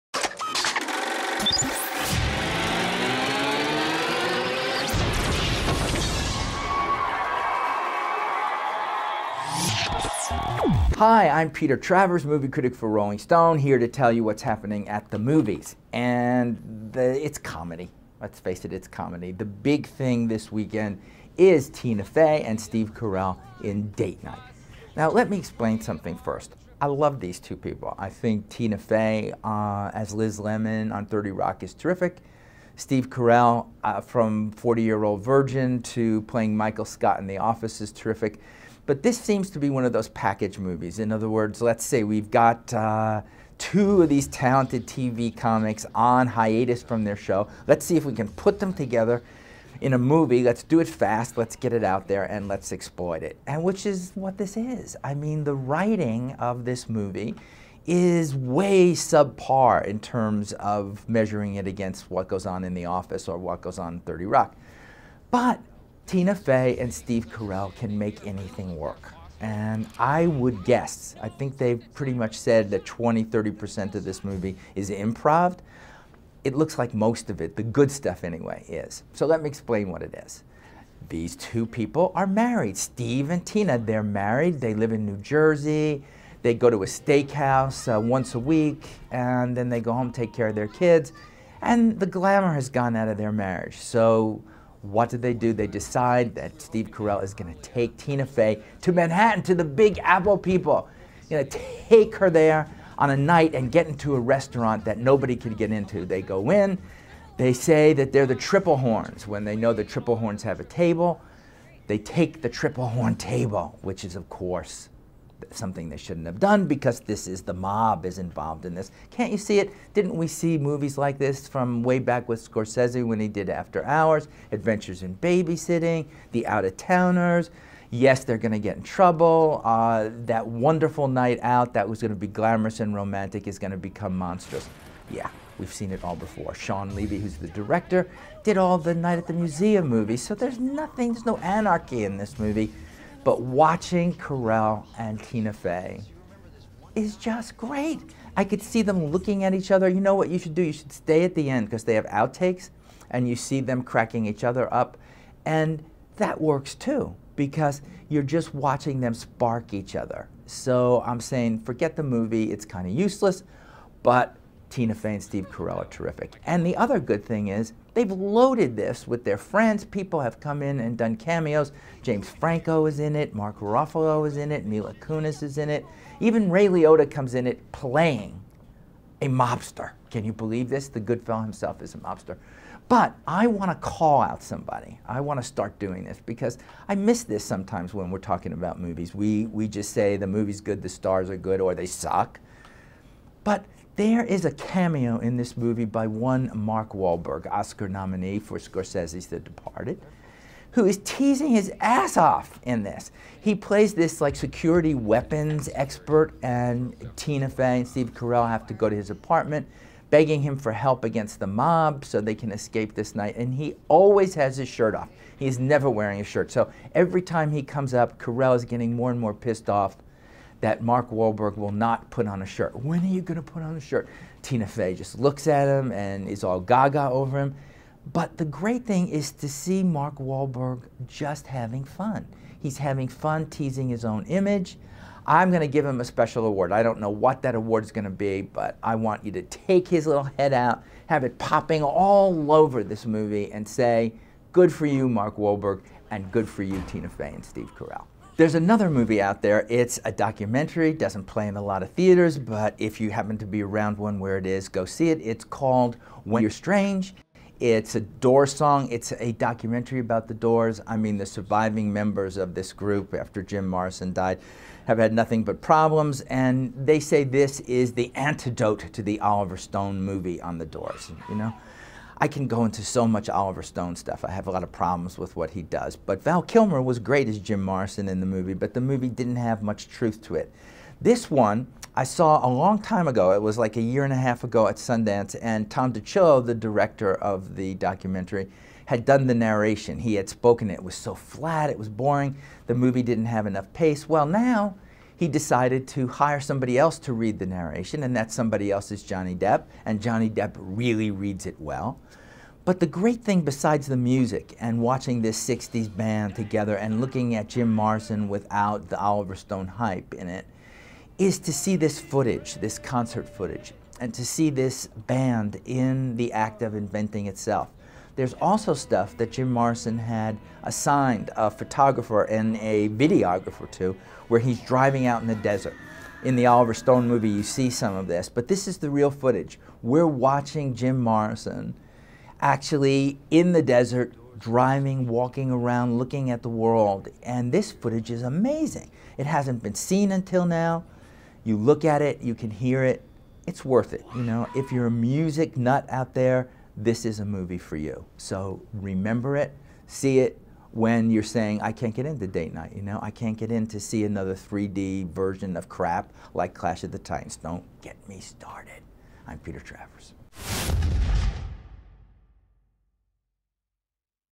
At the Movies with Peter Travers: 'Date Night' and 'When You're Strange'
This week in At the Movies, Rolling Stone's Peter Travers takes you on a Date Night with a pair of NBC Thursday Night marquee names: The Office's Steve Carell and 30 Rock's Tina Fey. The two titans of comedy play a suburban married couple that have lost the spark in their marriage until a case of mistaken identity have them on the run from mobsters in New York City. You've seen the plot before in movies like Blind Date and After Hours, and much of Date Night is sub par, but Carell and Fey's talents shine in the film.